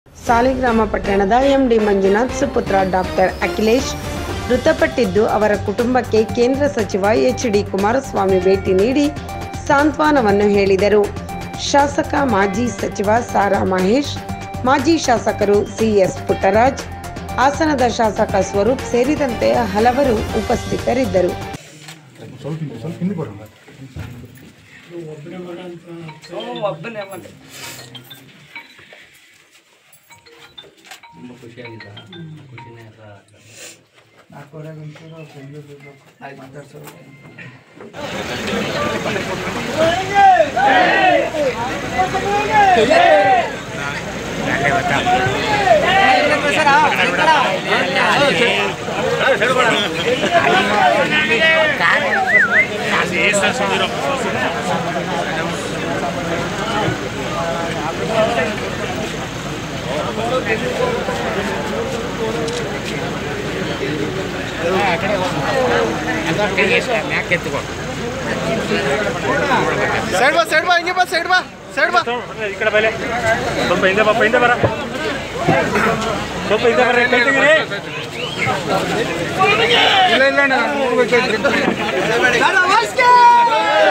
ಎಂ ಡಿ ಮಂಜುನಾಥ್ ಸುಪುತ್ರ ಡಾಕ್ಟರ್ ಅಖಿಲೇಶ್ ಮೃತ
ಸಾಲಿಗ್ರಾಮ ಪಟ್ಟಣದ ಎಂ ಡಿ  ಮಂಜುನಾಥ್  ಸುಪುತ್ರ ಡಾಕ್ಟರ್  ಅಖಿಲೇಶ್  ಮೃತ ಪಟ್ಟಿದ್ದು ಅವರ ಕುಟುಂಬಕ್ಕೆ ಕೇಂದ್ರ ಸಚಿವ ಎಚ್ ಡಿ ಕುಮಾರಸ್ವಾಮಿ ಭೇಟಿ ನೀಡಿ ಸಾಂತ್ವಾನವನ್ನು ಹೇಳಿದರು. ಶಾಸಕ ಮಾಜಿ ಸಚಿವ ಸಾರಾ ಮಹೇಶ್. ಮಾಜಿ ಶಾಸಕರು ಸಿ ಎಸ್ ಪುಟ್ಟರಾಜ್. ಹಾಸನದ ಶಾಸಕ ಸ್ವರೂಪ  ಸೇರಿದಂತೆ ಹಲವರು ಉಪಸ್ಥಿತರಿದ್ದರು